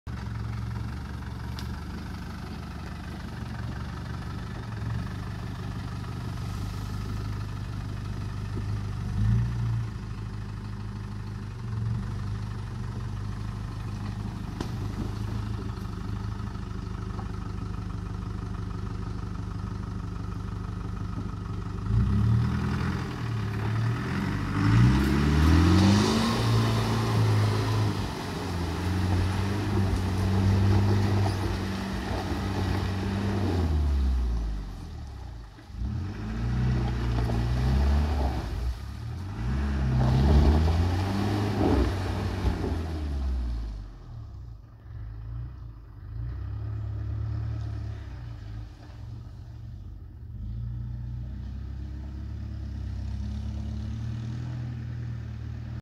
Not getting stuck today! Discovery 1 200tdi
Some ruts in the mud but not today